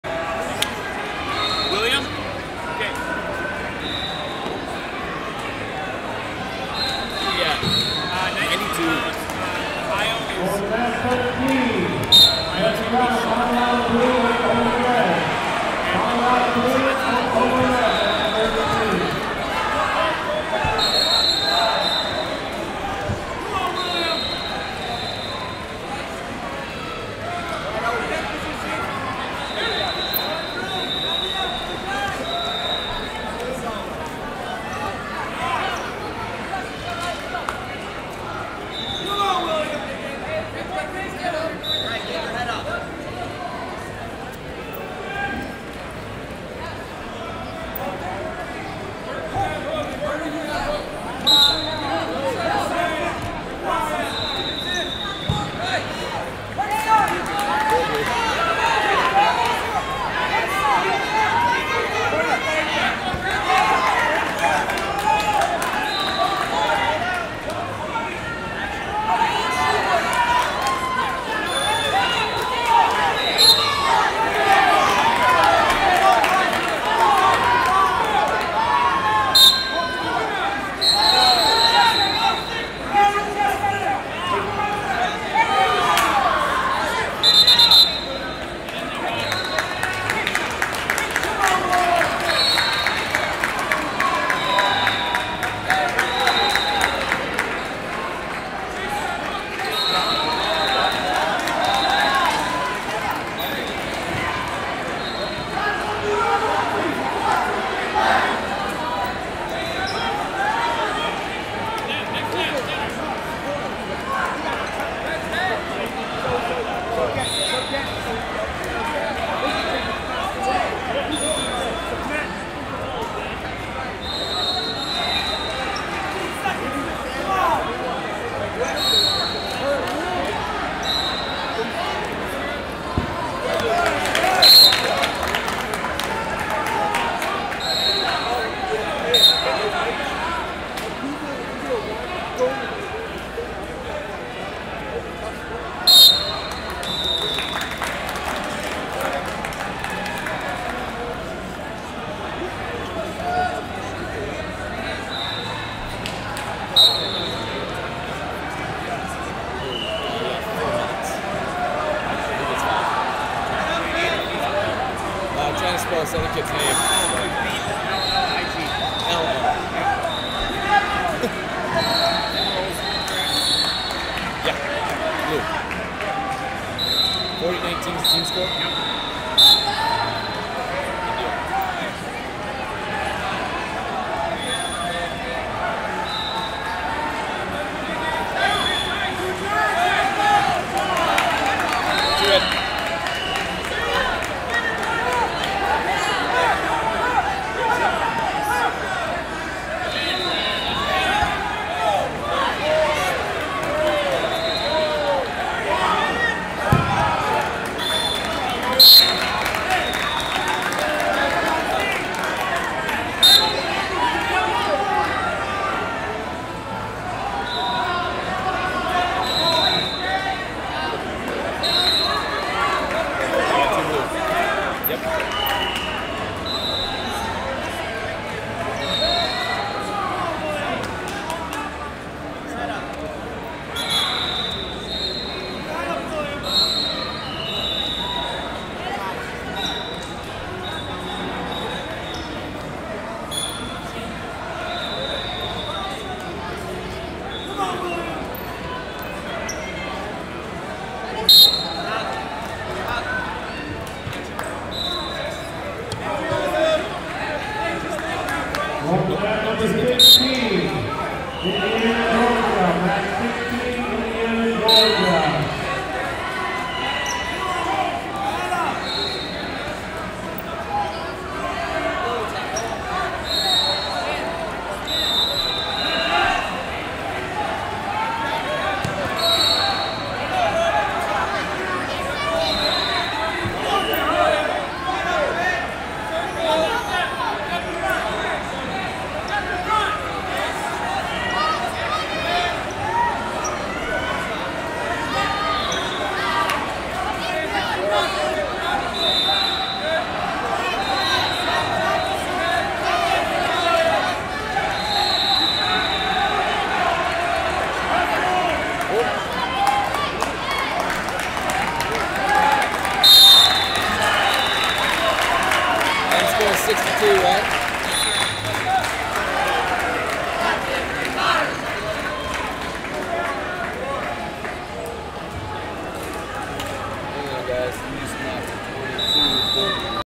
92 Lbs Round 2 (8 Team) - William Schork, Ohio Vs Kolten Bollig, Wisconsin 4a2c
Flosports-partner-access/2023 14U National Duals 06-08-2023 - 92 Lbs Round 2 (8 Team) - William Schork, Ohio Vs Kolten Bollig, Wisconsin 4a2c